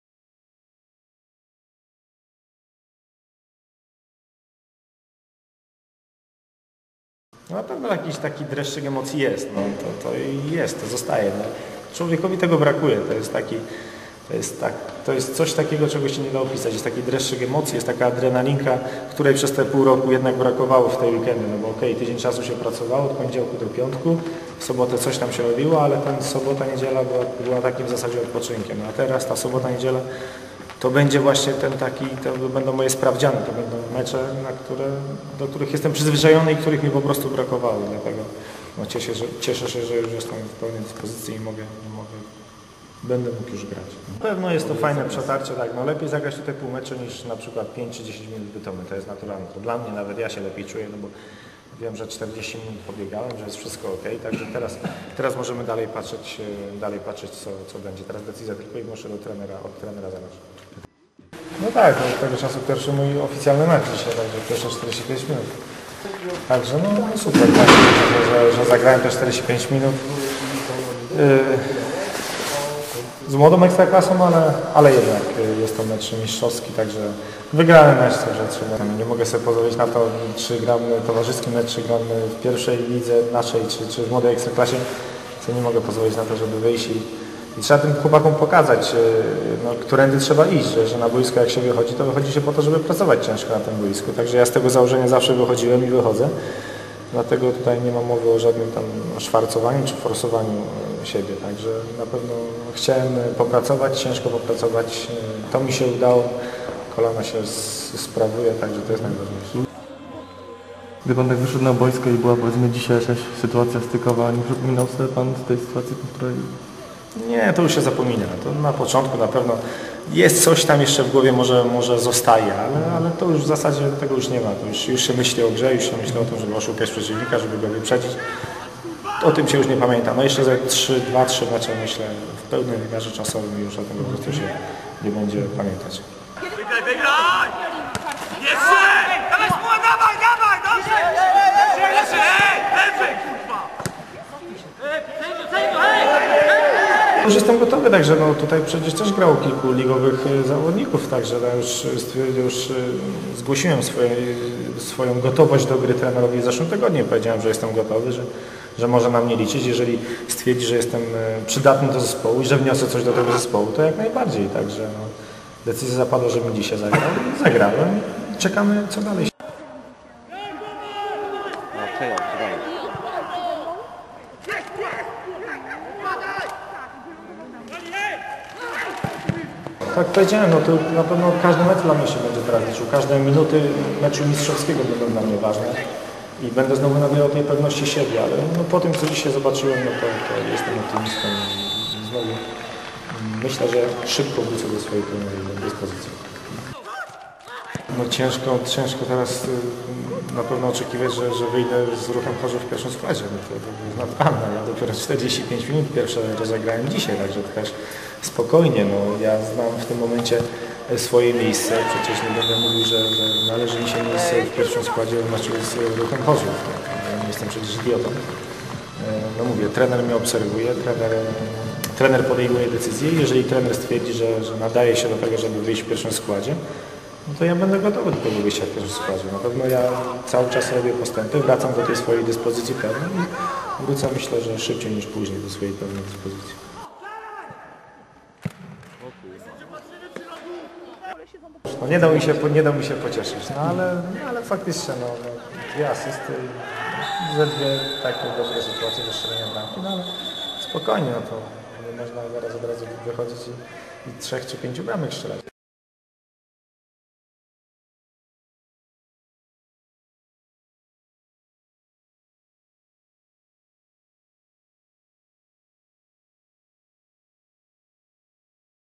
Andrzej Niedzielan wraca do gry!
Wywiad z Andrzejem Niedzielanem po jego pierwszym meczu po kontuzji wraz z sytuacjami zawodnika z tego meczu